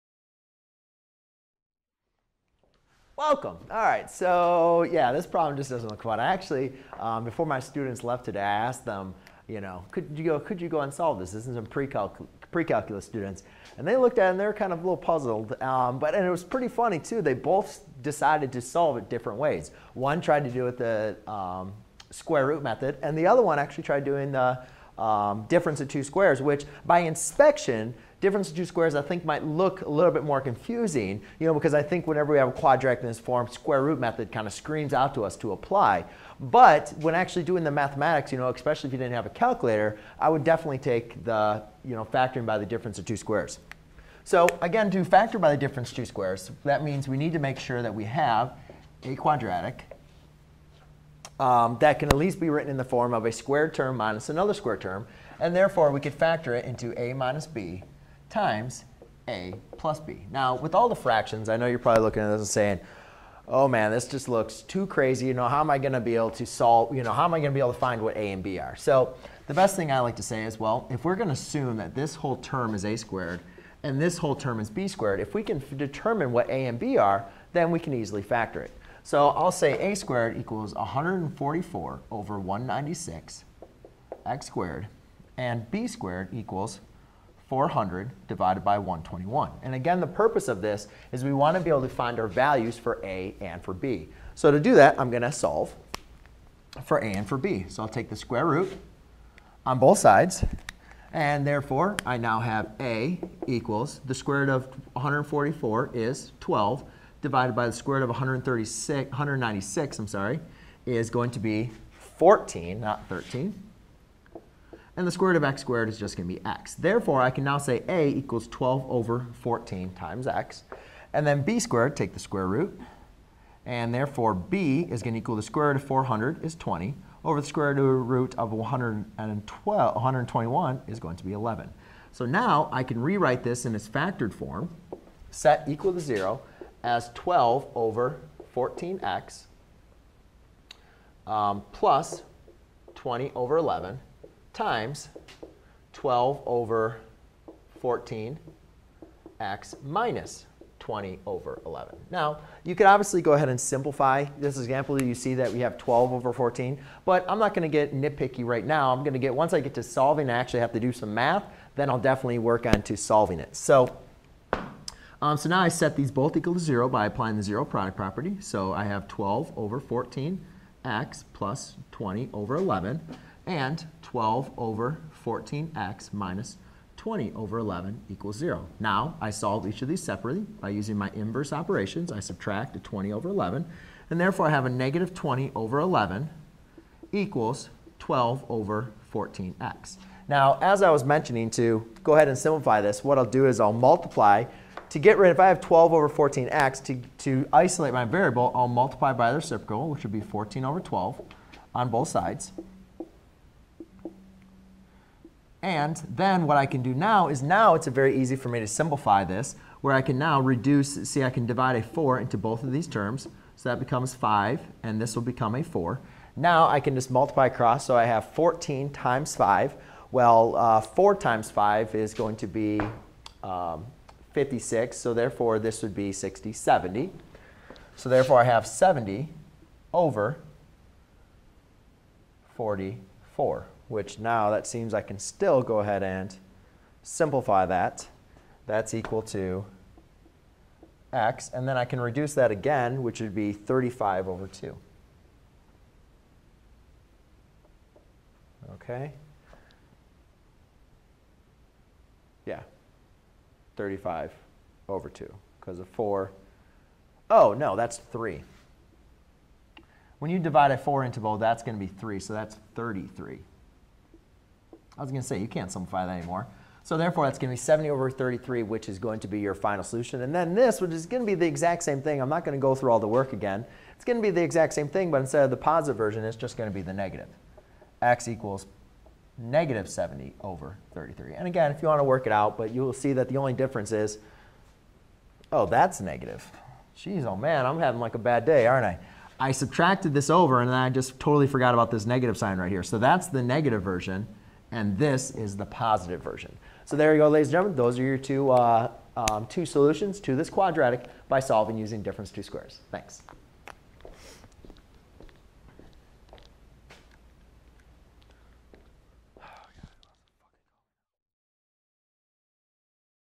With Fractions, solve using the difference of two squares
👉Learn how to solve quadratic equations by factoring the difference of two squares.  We can identify the difference of two squares but looking for binomials that have square terms. Difference of two square quadratic equations is of the form (x^2 - a^2) = 0, where a^2 is a perfect square. The factored form of a difference of two squares quadratic equation is of the form (x - a)(x + a), where a is the square root of a^2. When we have factored the difference of two squares, we then apply the zero product property to solve the quadratic equation.